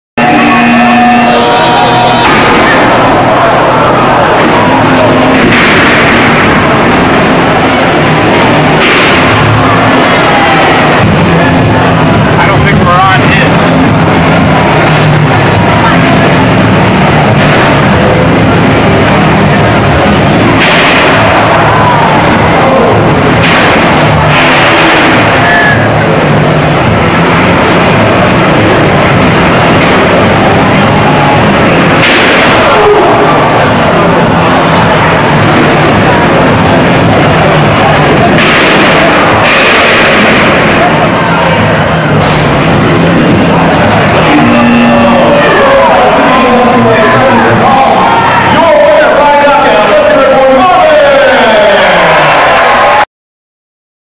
RoboGames 2005: Marven 3.0 vs Ziggy
The big wedge armed with a drum of Marvin 3.0 takes on the launching beast known as Ziggy in this BattleBots esque bout.